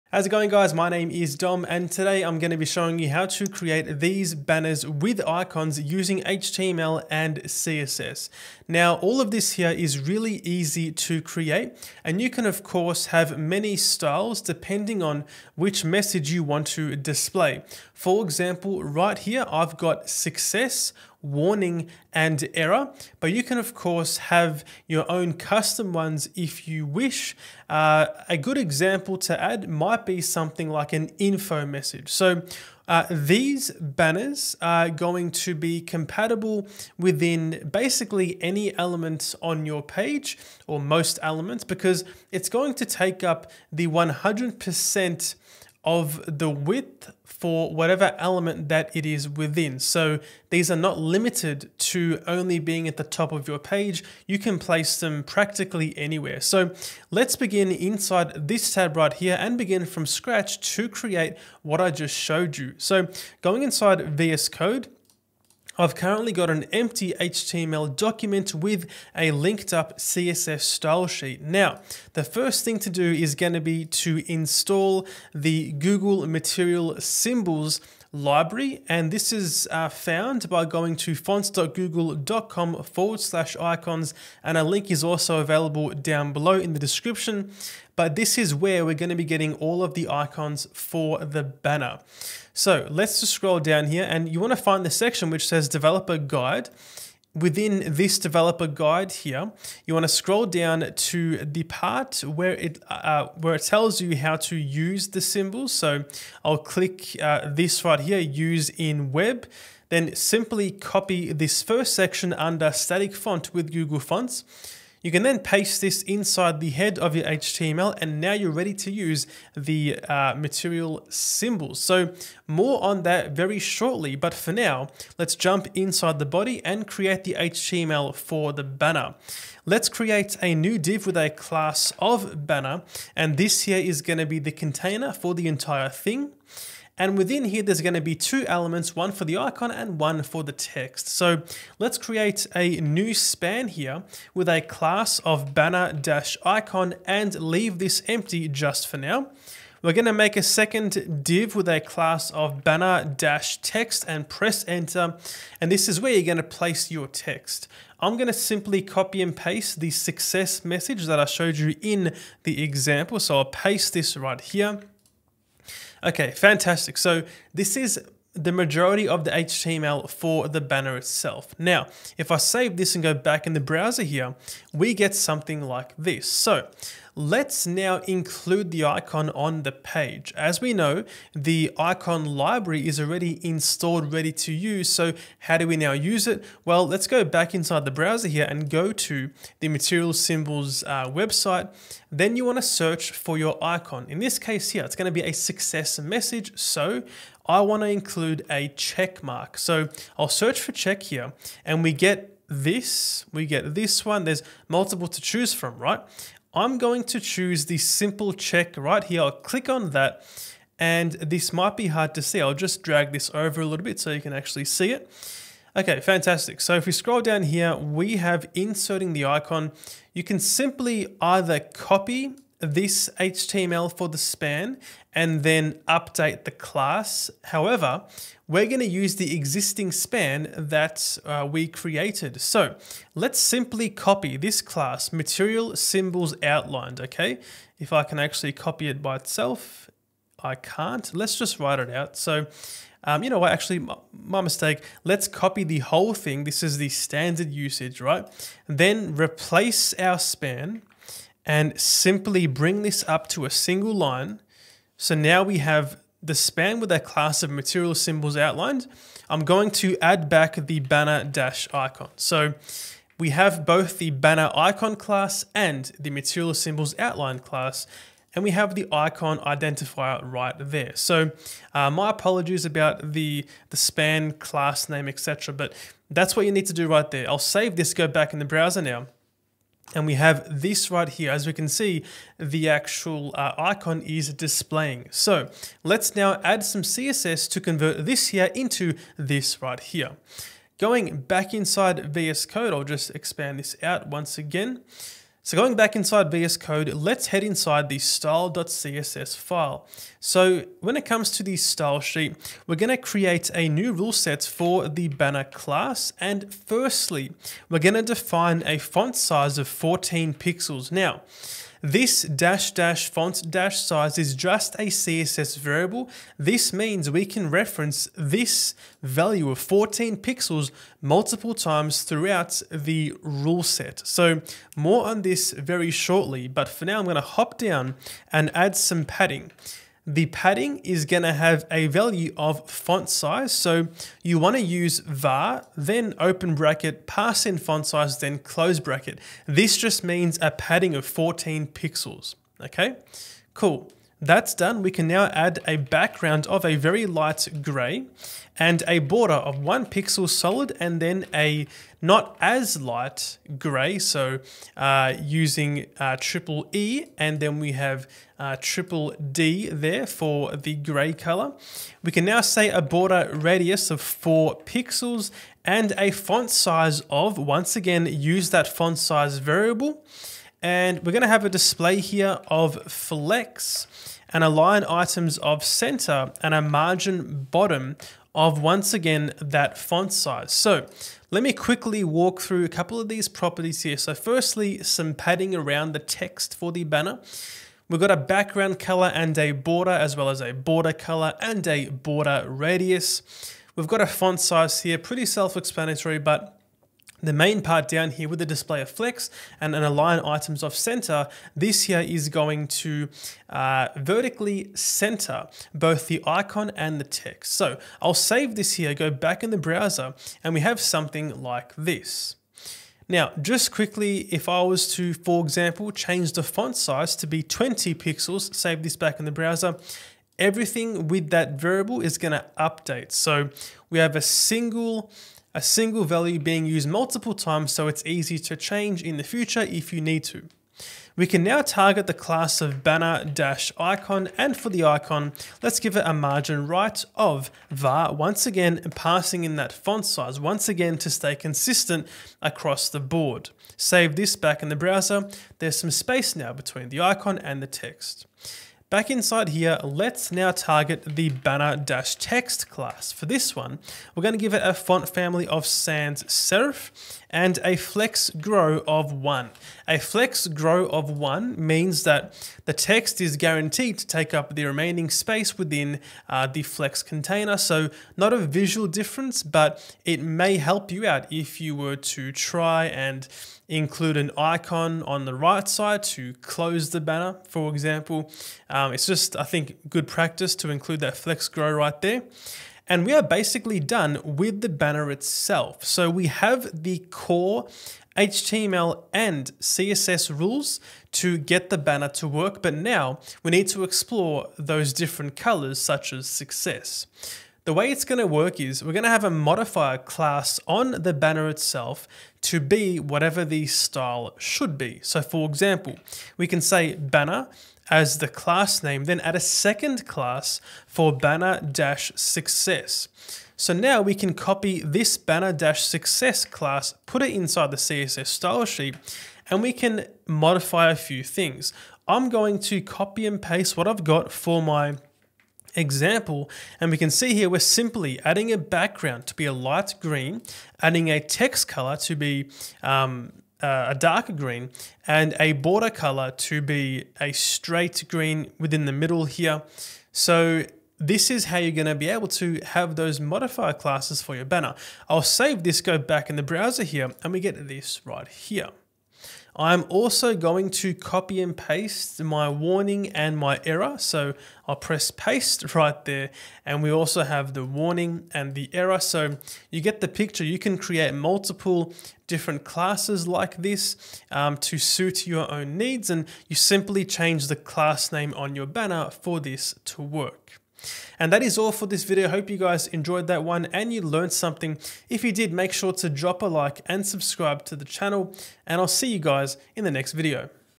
How to Add Banners with Icons to Your Website - HTML & CSS Tutorial
Using HTML & CSS, it's really easy to create reusable banners for your website or project. These can be used practically anywhere on your web page and come in different colours depending on your use case! 🎨